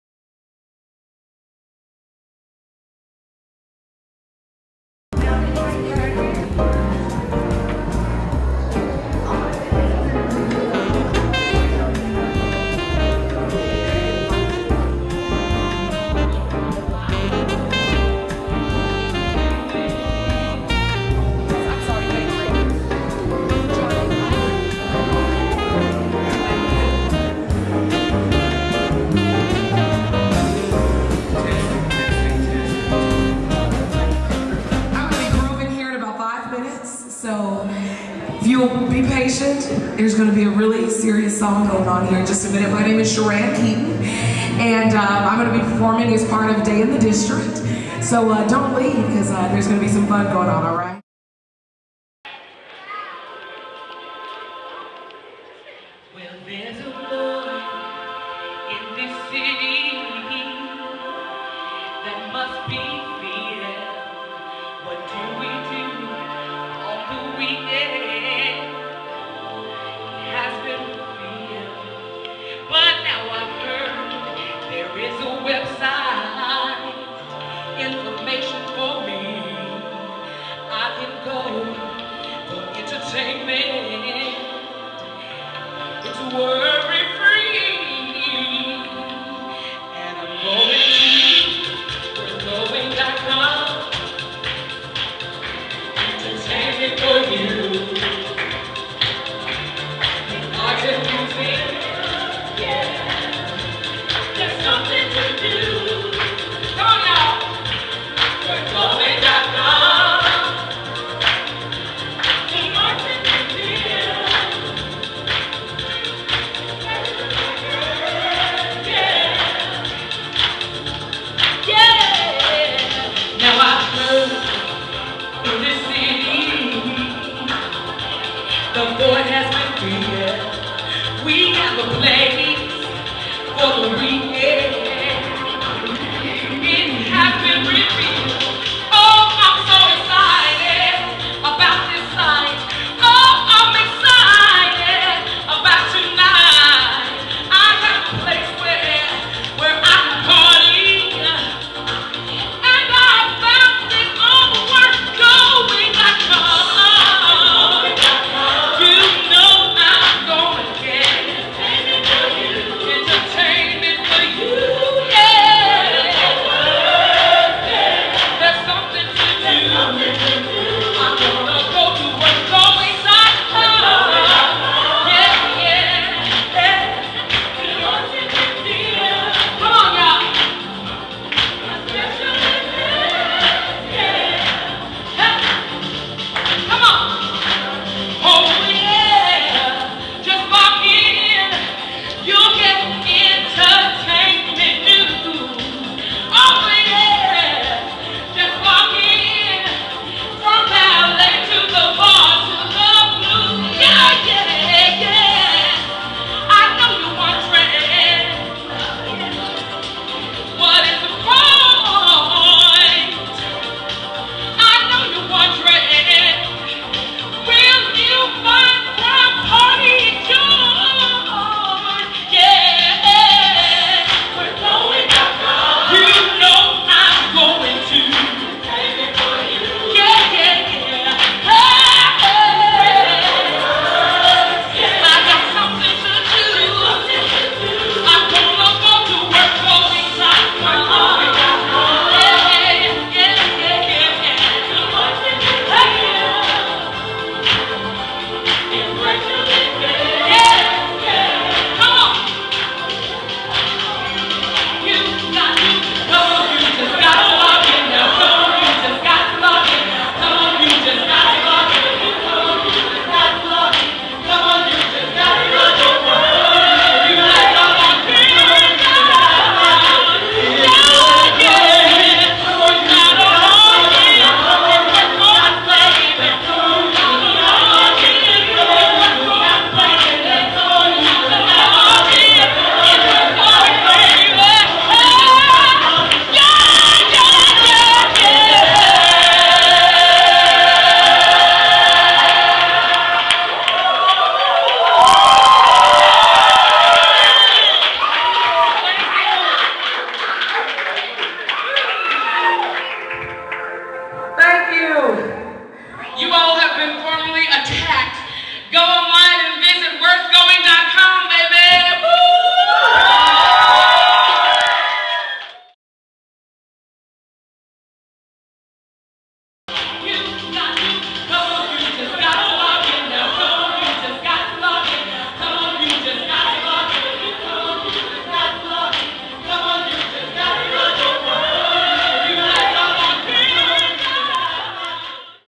Going Gospel
ART ATTACK X: It started out as a simple performance during "Day in the District" -- Fort Worth's free event in the Cultural District.

The performer was local celebrity Sheran Keyton, but it didn't take long for some of her closest friends to join in the performance.

It was a lyrical flash mob!

Along with the Carter High School choir, Samaritan House choir and DVA Productions, Keyton wrote and performed an original song about WorthGoing.com.

Reminder: Voting starts on Oct. 6